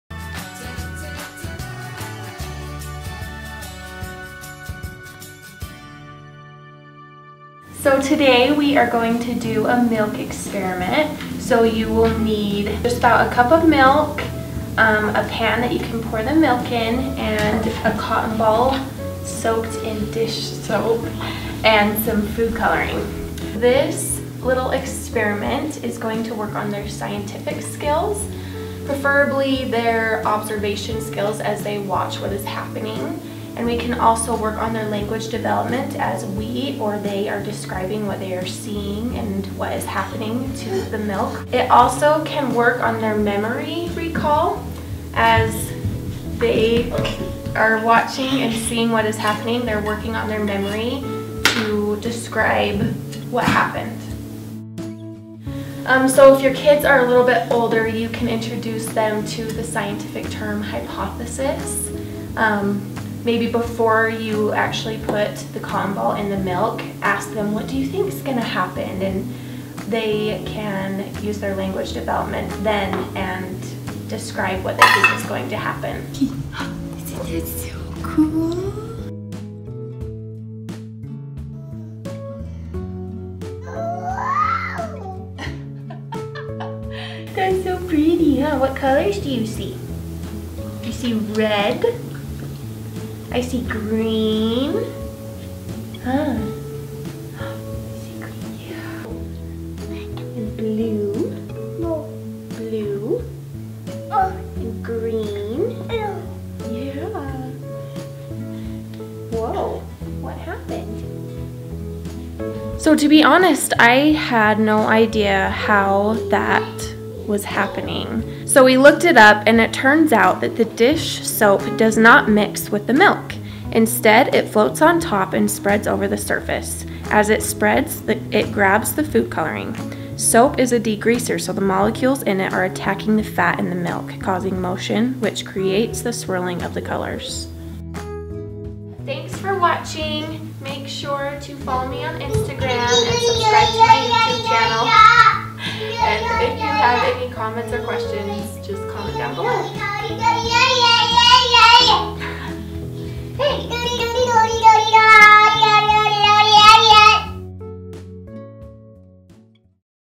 Milk Experiment for Kids- Milk and Food Coloring
✨Milk experiment✨
This is such a great little science experiment you can do with your littles. This helps with basic science skills, language development, color recognition, and memory recall. And don’t wear freaking white pants while doing this! 😉